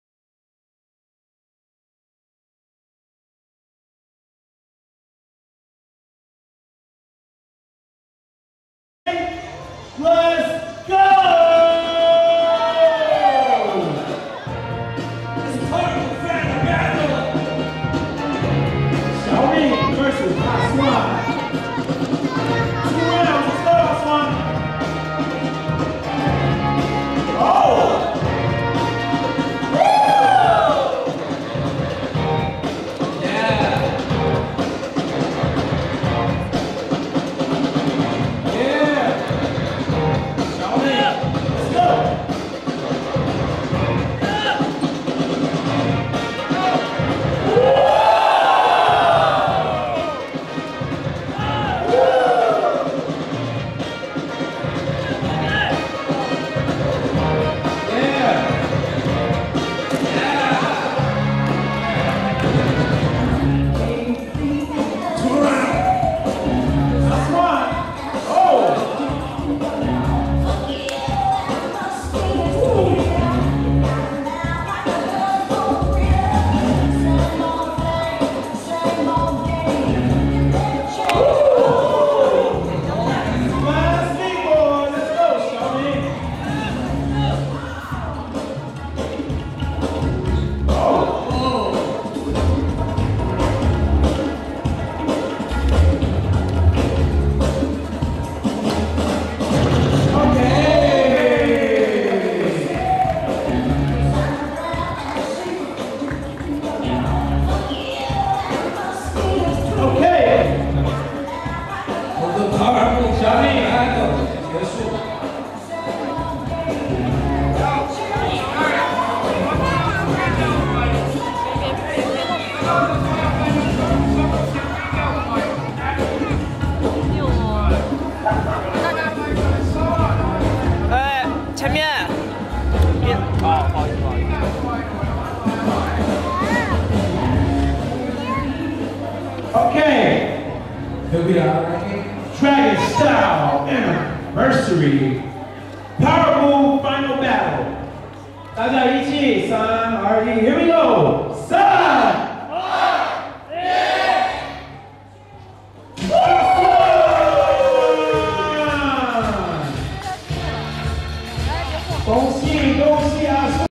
Powermove Final | Power Sour VS Xiao Ming @ Dragonstyle 11th Anniversary | LB-PIX
Dragonstyle 11th Anniversary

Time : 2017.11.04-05
Address : Beijing,China

Event : B-Girl 1vs1
        Powermove 1vs1
        Kid 1vs1
        2vs2 Battle
        Crew Battle
        Footwork 1vs1
        Top Rock 1vs1

Judge : Milky
        EZ Mike
        Maurizio
        Pepito
        Hoya
        Physicx

MC : Killa4nia
     Man of God
     Bird

DJ : Timber
     LDC
     DSK

master film&drone film
*LB PIX SNS

drector&motiongraphic&backstage film
*ChanPD SNS